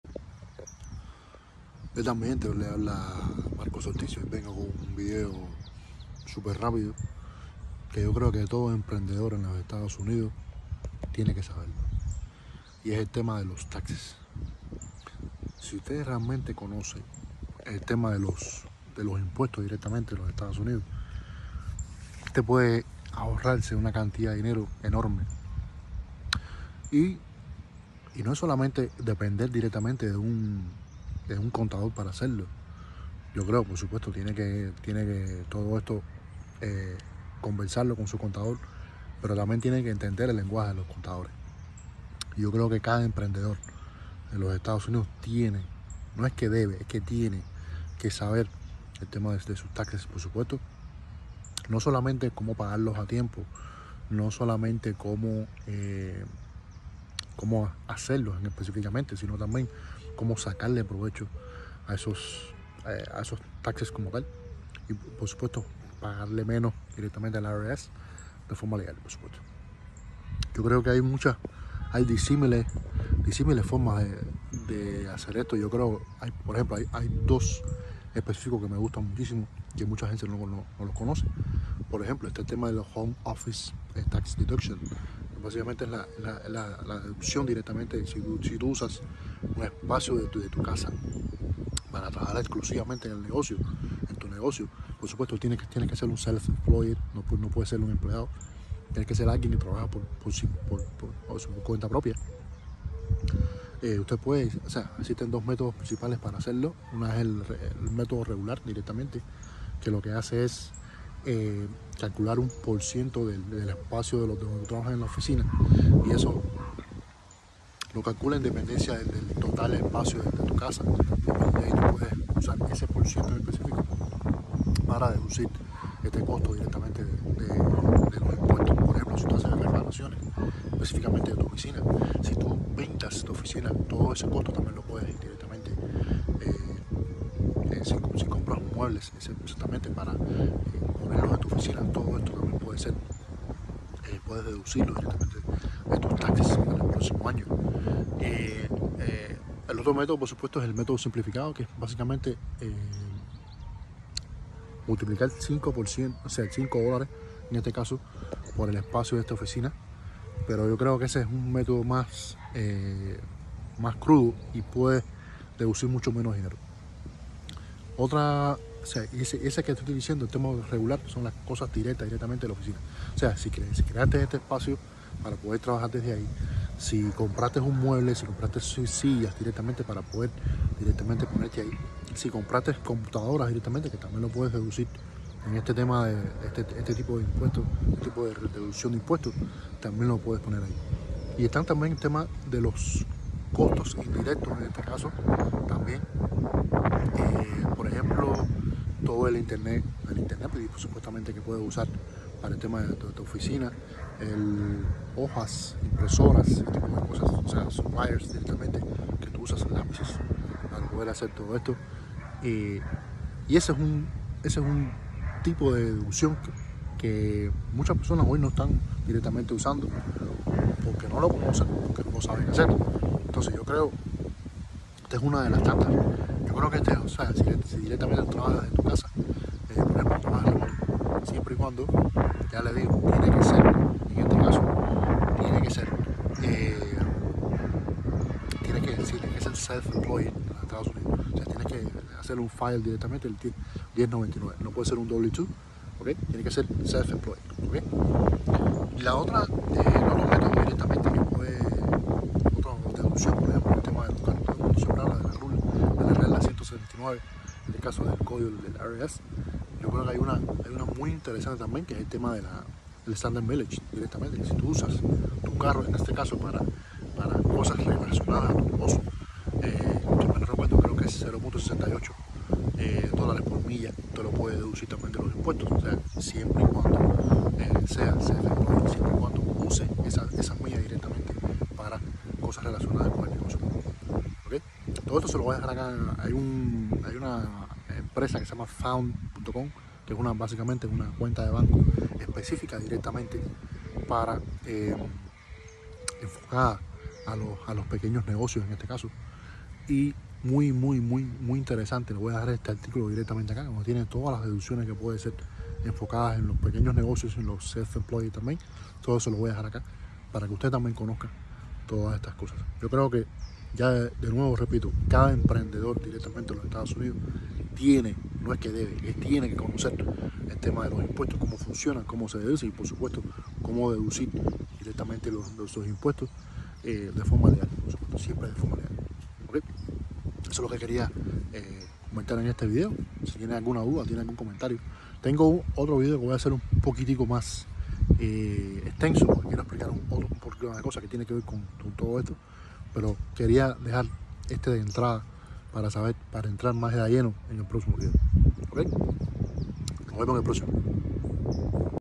Todo emprendedor en USA debe conocer esto: Taxes, IRS, Deducciones y más
Si hay algo que cada emprendedor en Estados Unidos debe conocer es como hacer los taxes, o al menos entenderlos para cuando estes conversando con tu contador